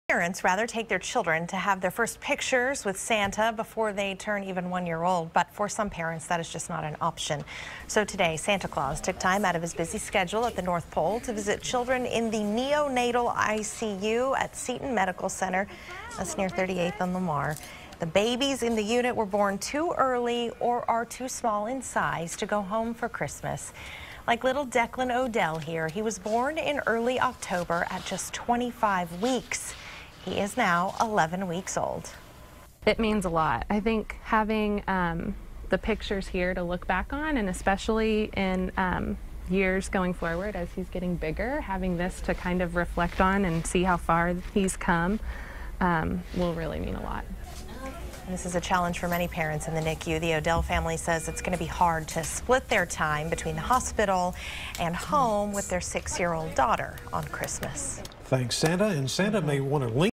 Santa makes a visit to babies at the NICU at Seton Medical Center
Santa Claus stopped by the Neonatal ICU at Seton Medical Center Austin to bring holiday cheer to parents and their babies born too early or too small in size to go home for Christmas